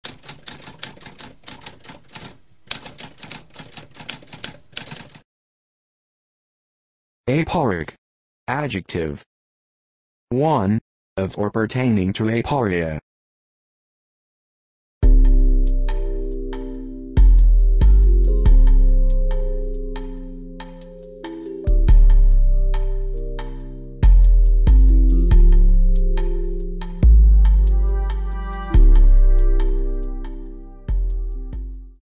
What does aporic mean?
A spoken definition of aporic.

Intro Sound:
Typewriter - Tamskp